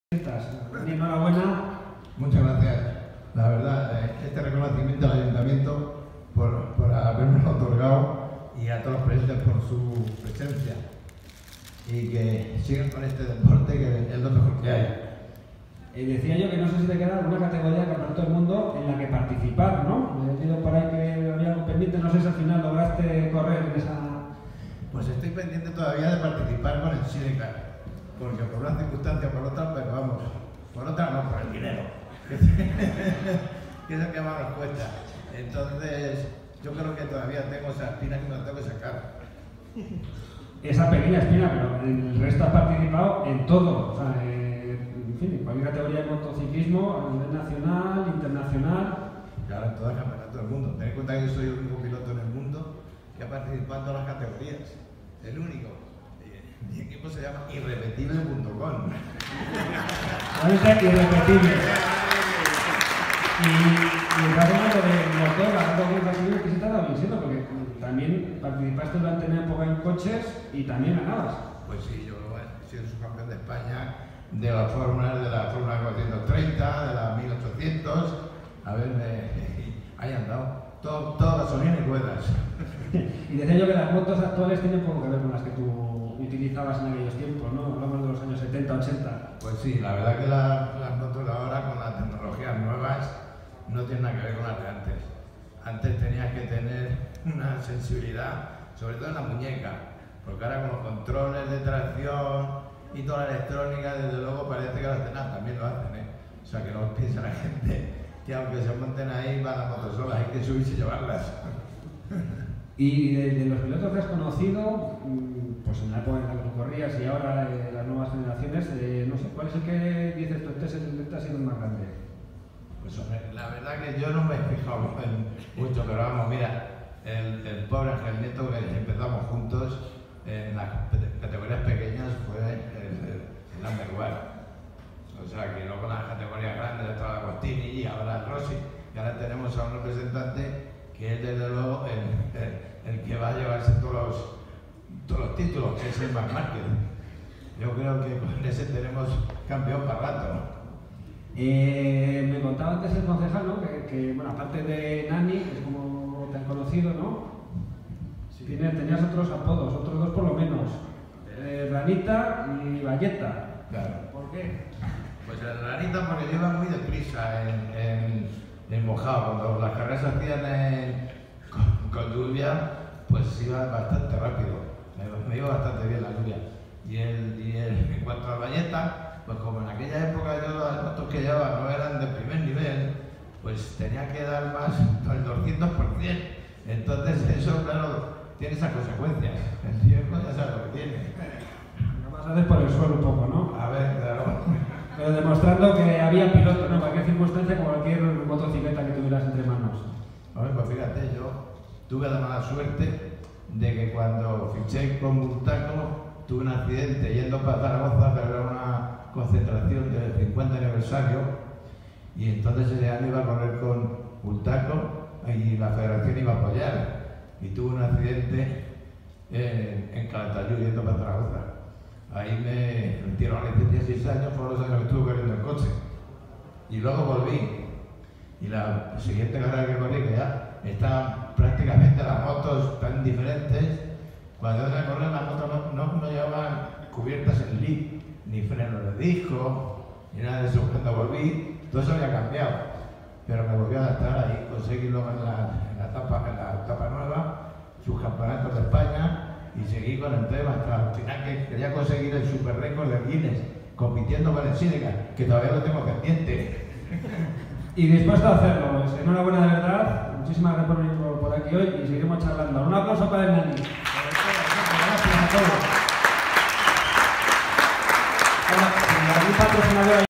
Fernando González de Nicolás, recoge su premio
VI Gala del Deporte en Sigüenza.